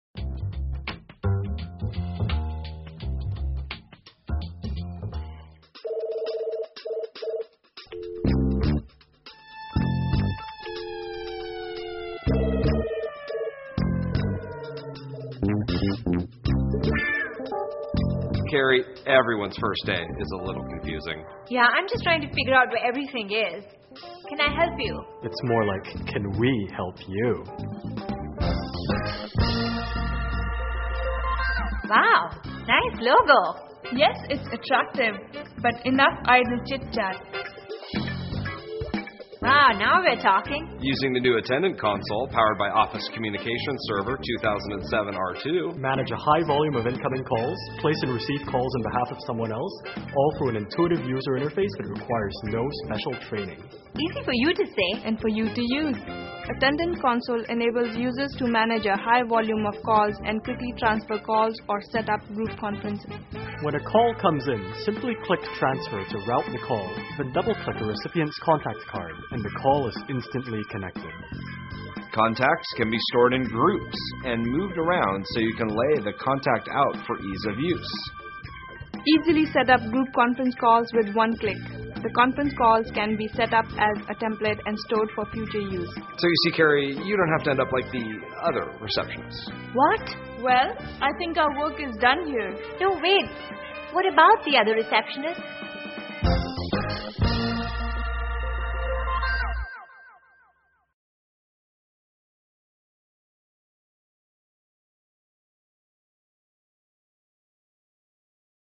Office Communications Server 2007 R2 Attendant
How Microsoft can change your office with the Office Communications Server 2007 R2.